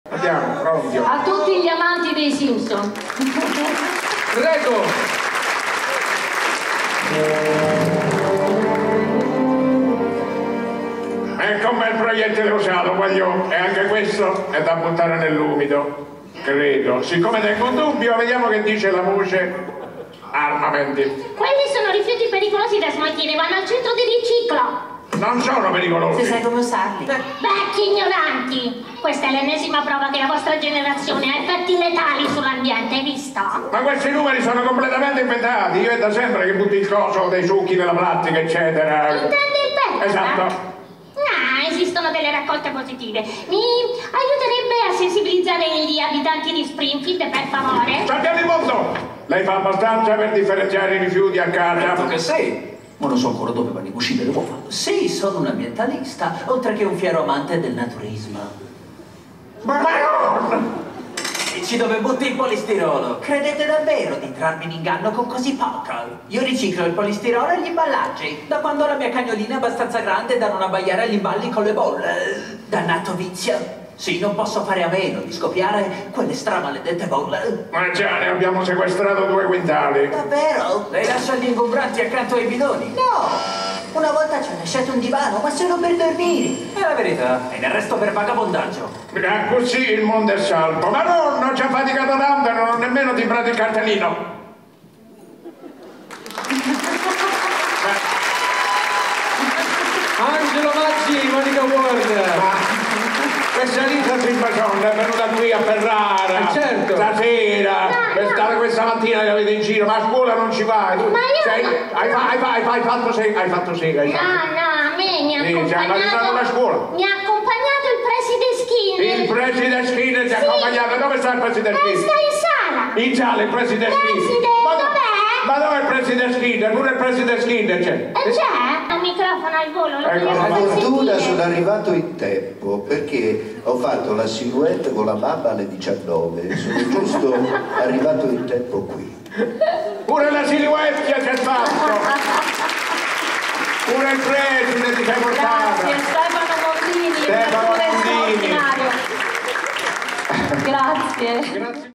Monica Ward e Angelo Maggi | Musicfilm Festival 2022
Come si può rispettare l'ambiente? 🌱
Ce lo hanno spiegato le voci di Lisa Simpson e il Commissario Winchester, durante l'ultima edizione di musicfilmfestival, che hanno trovato una nuova guida per riciclare correttamente: Monica Ward e Angelo Maggi.

Grazie anche a Stefano Mondini (Preside Skinner) e a Gli Acchiappafilm.